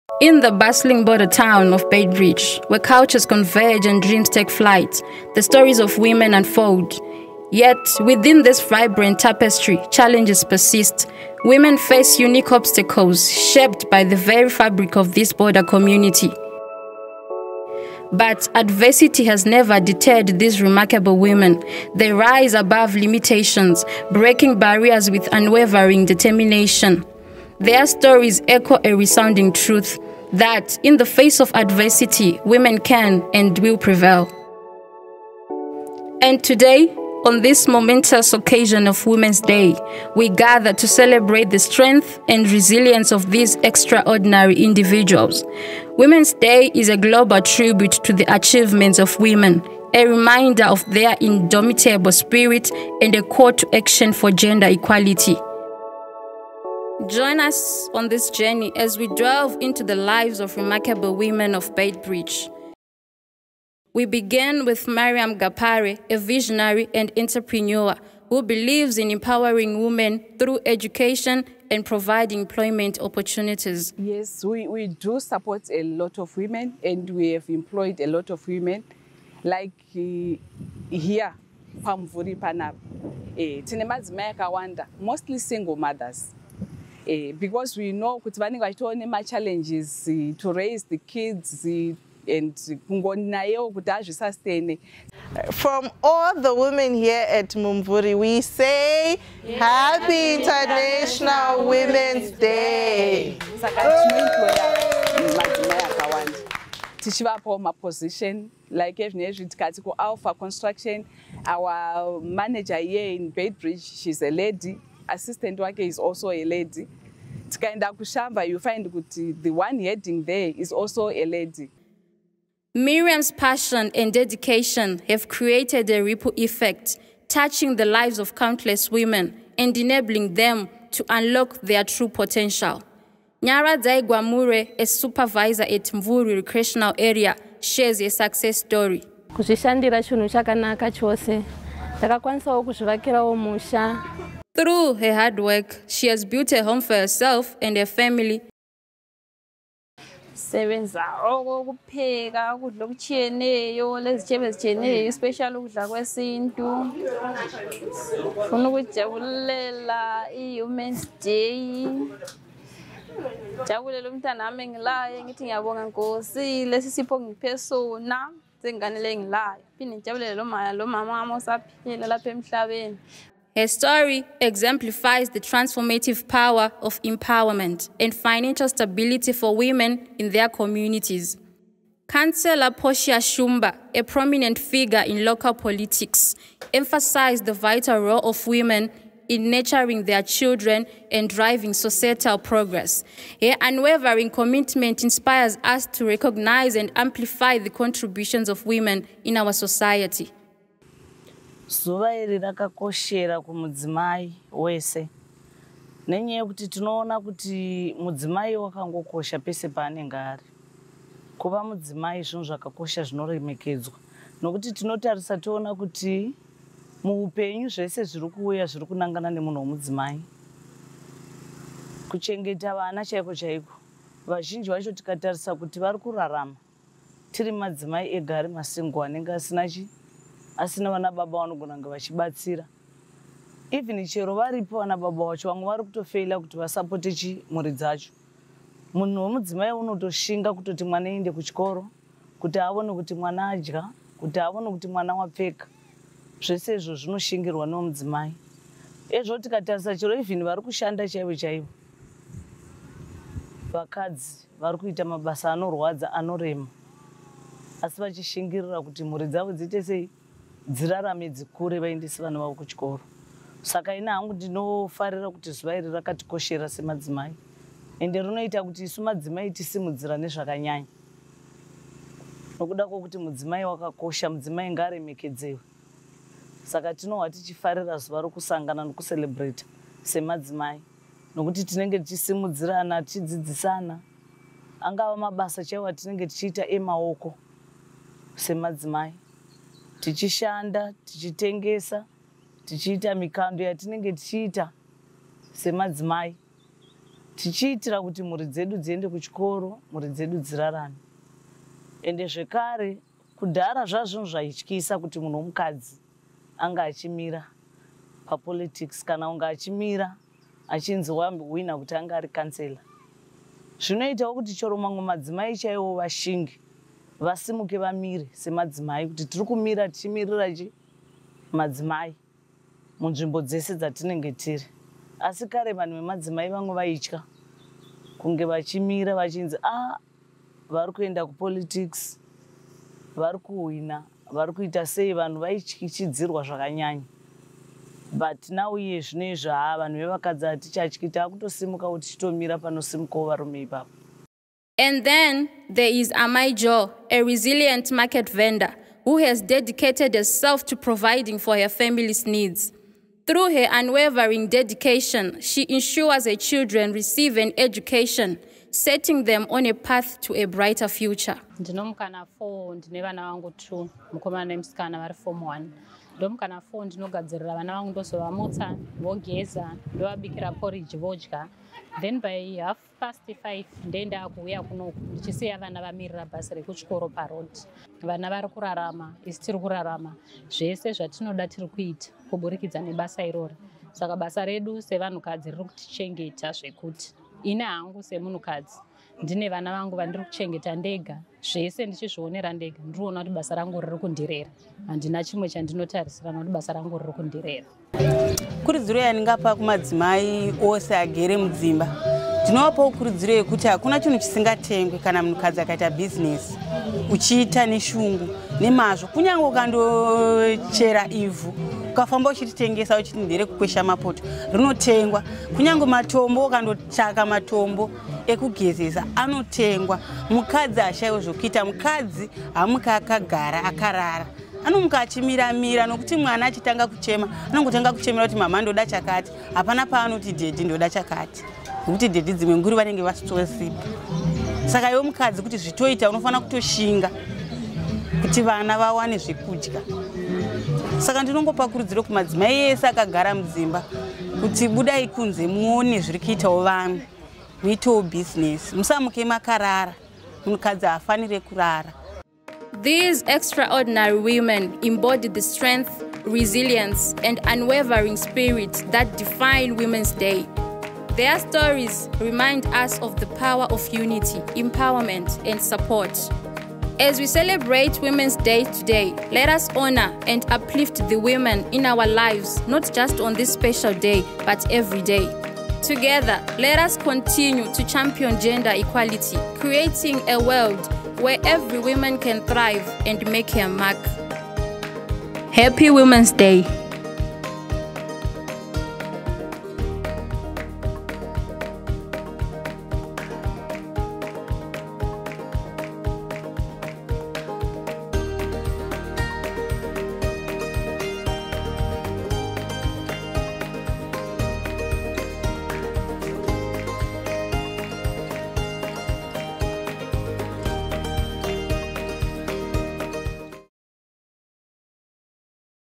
Her Powers - Stories of Strength and Success - Beitbridge Women
Celebrate Women's Day with us as we proudly present "Her Powers: Stories of Strength and Success," a captivating documentary that takes you on a remarkable journey through the lives of extraordinary women in Beitbridge.

In this powerful film, we delve deep into the resilient spirit and indomitable will of women who have overcome adversity, shattered stereotypes, and triumphed against all odds. From entrepreneurs and activists to artists and community leaders, these women have harnessed their unique abilities, unleashing their true potential to make a lasting impact on their families, communities, and society as a whole.

Shot on location in Beitbridge, this documentary showcases the vibrant and diverse stories of women who have defied societal expectations and emerged as beacons of inspiration. Through personal interviews and intimate footage, we witness their challenges, victories, and the transformative power of their journeys.

Join us as we celebrate these unsung heroes, shedding light on their remarkable achievements and the pivotal roles they play in shaping a more inclusive and equitable world. Their stories will leave you inspired, motivated, and eager to support the empowerment of women in your own community.

 / ziyahmedia